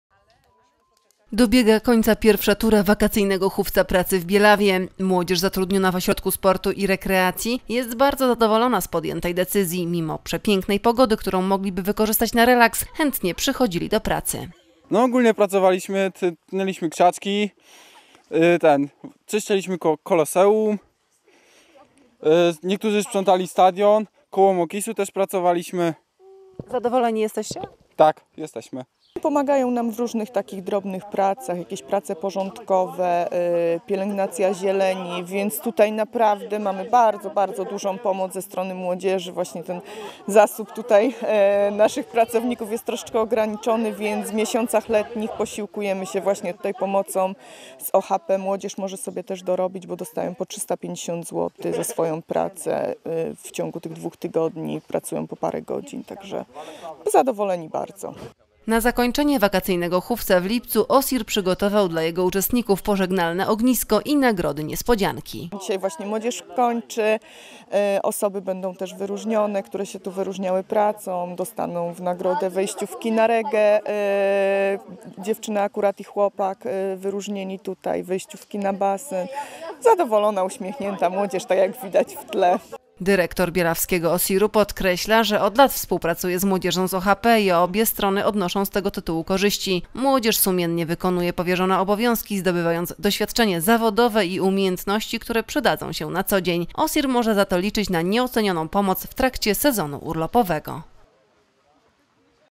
WAKACYJNY HUFIEC PRACY
Dobiegła końca pierwsza tura Wakacyjnego Hufca Pracy w Bielawie. Młodzież zatrudniona w Ośrodku Sportu i Rekreacji jest bardzo zadowolona z podjętej decyzji. Mimo przepięknej pogody, którą mogliby wykorzystać na relaks, chętnie przychodzili do pracy.